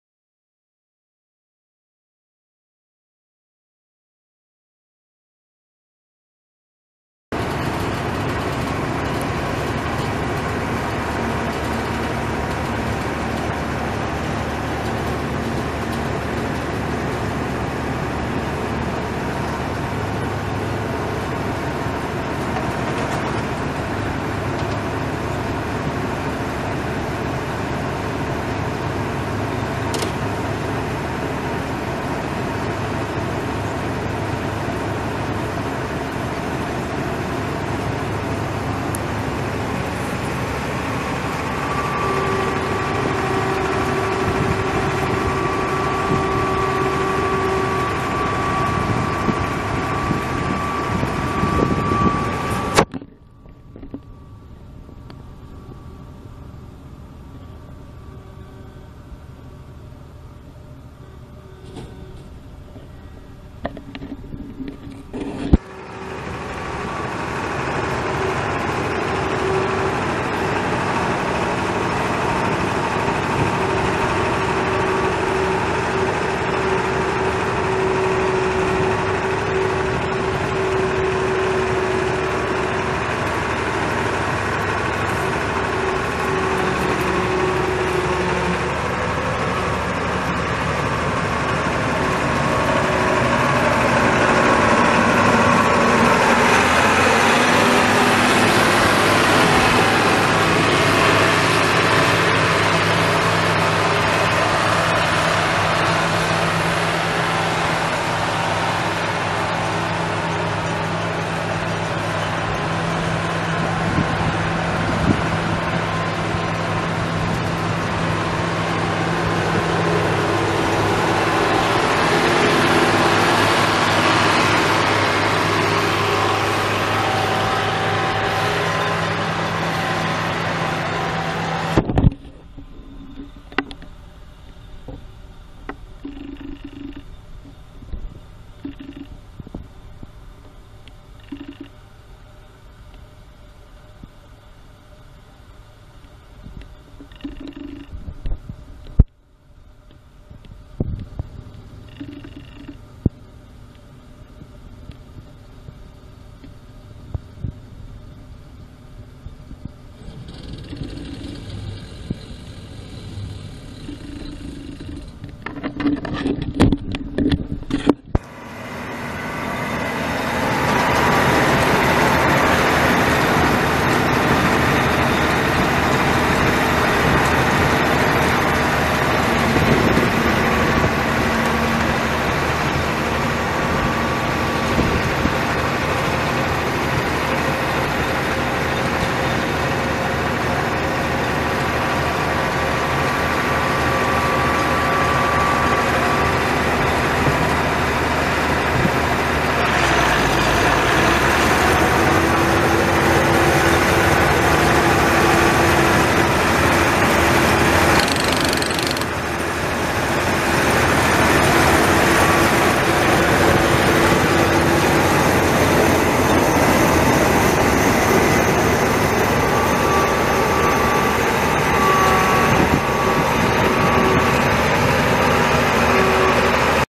Gleaner L combine for sale at auction | bidding closes June 6, 2018
Gleaner L combine
672 engine hours on meter
Allis Chalmers six cylinder turbo diesel engine
Hydrostatic transmission
2F-2R gears
Feederhouse
Variable speed
AC and heat
Suspension seat
Milo fingers
Straw chopper
Work lights
Gleaner rigid head
20'W
Bat reel
Hydraulic reel height control
Serial LM21157
23.1-34 front tires
11.00-16 rear tires
Serial L4382HY
It's located in Dighton, KS, and we're selling it to the highest bidder regardless of price.